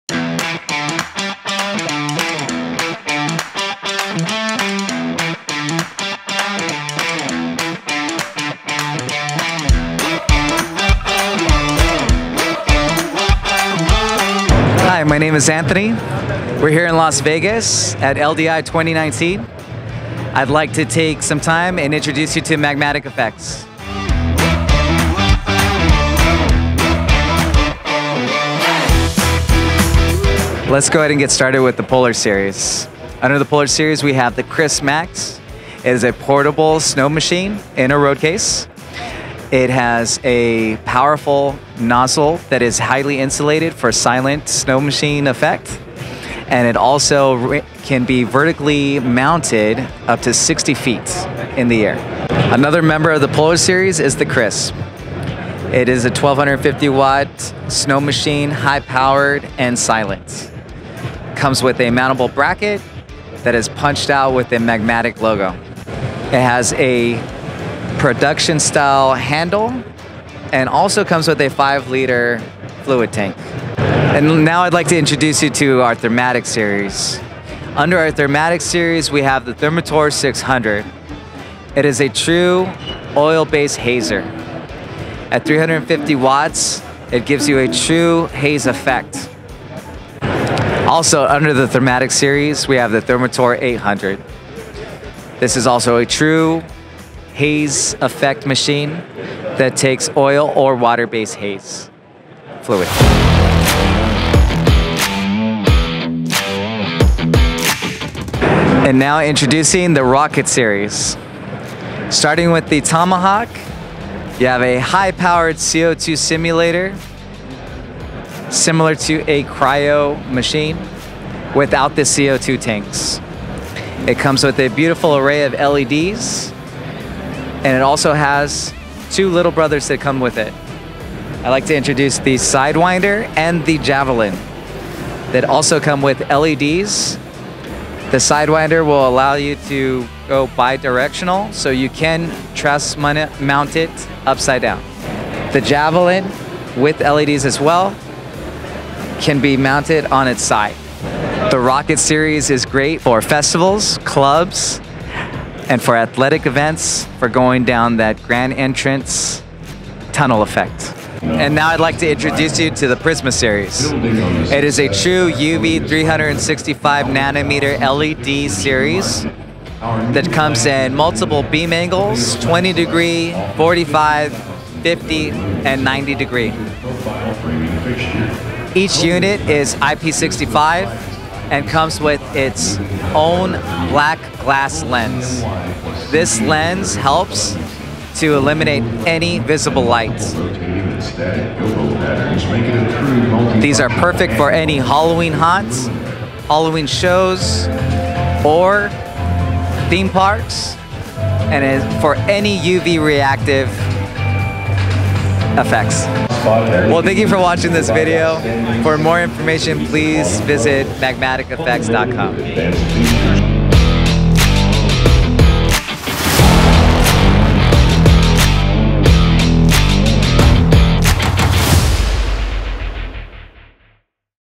MAGMATIC at LDI2019
Introducing MAGMATIC! A new line of atmospheric effects products by Elation Professional.

Making its world debut at LDI2019, Anthony Velazquez introduces the various product lines for various applications which include haze machines, snow machines, CO2 cryogenic type simulated fog effect and UV lighting effects.